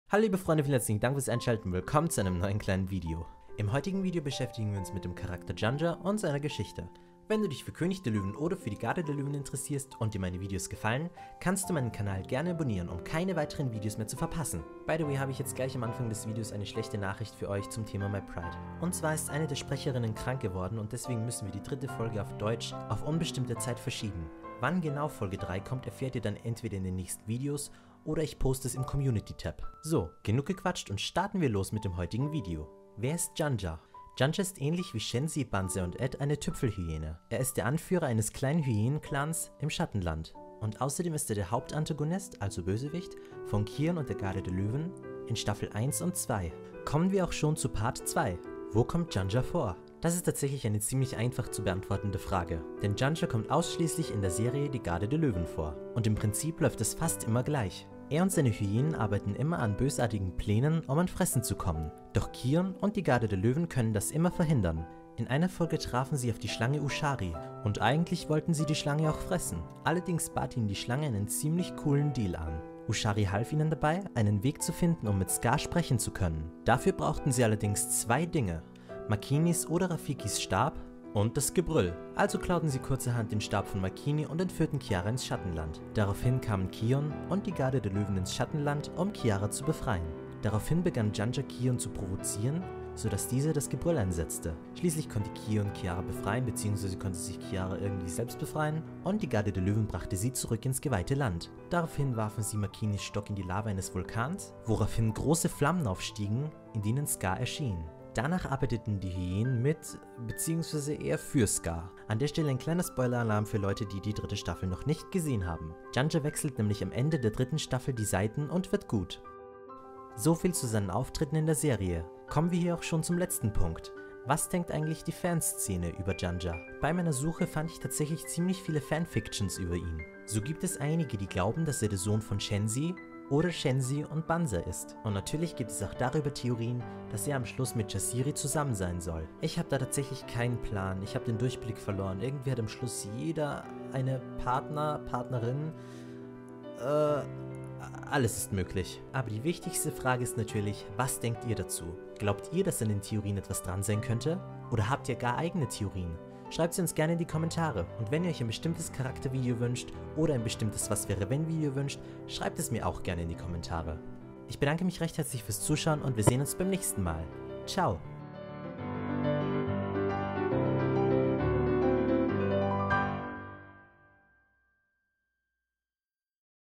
Alles über Janja |DerFlozi König der Löwen
Heute beschäftigen wir uns mal mit dem Charakter Janja :)

Gäste: Keiner
Im Video vorgekommene Elemente:
Quellen: